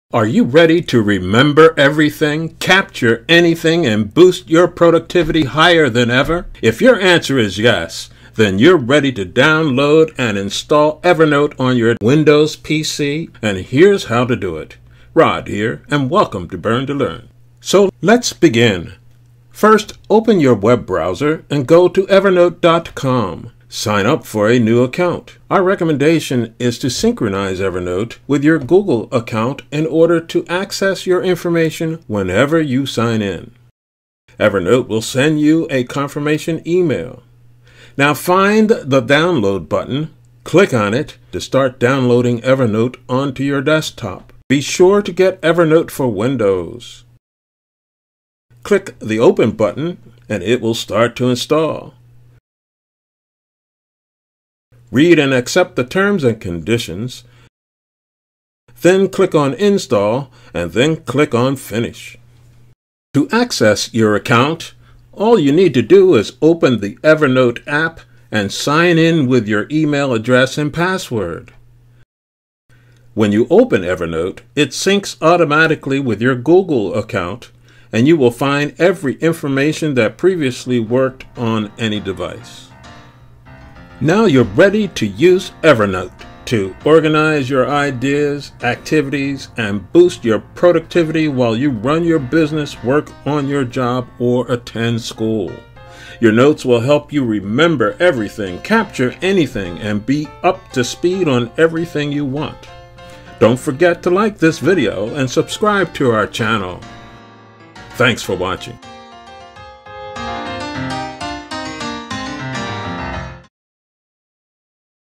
Evernote Windows INSTALLATION | EVERNOTE PC
This video will show you how to download and install EVERNOTE on your windows computer so that you can then remember everything, capture anything and boost your productivity higher than ever.
Learning how to use EVERNOTEFORWINDOWS is what this video is all about, be sure and checkout the entire playlist